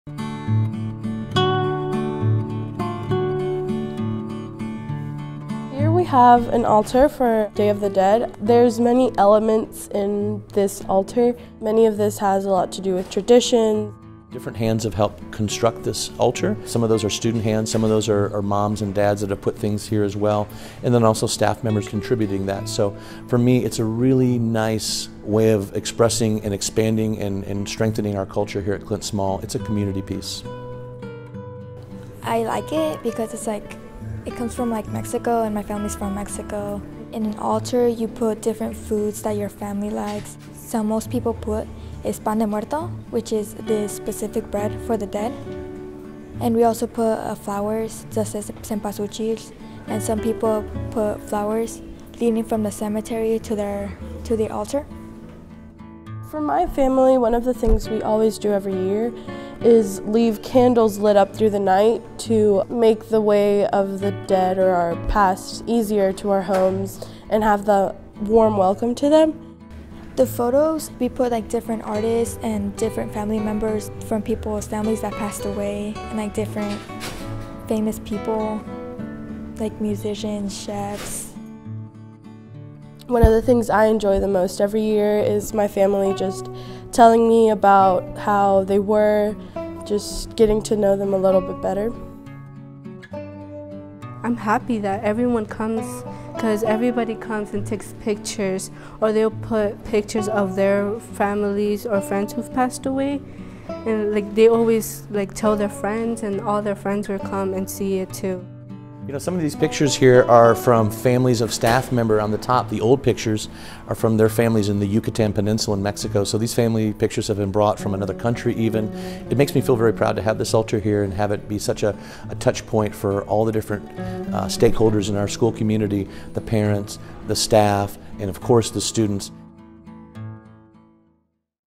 Día de los Muertos at Small Middle School
Students in the Dual Language program at Clint Small Middle School shared the tradition building of a Día de los Muertos altar with the Small community.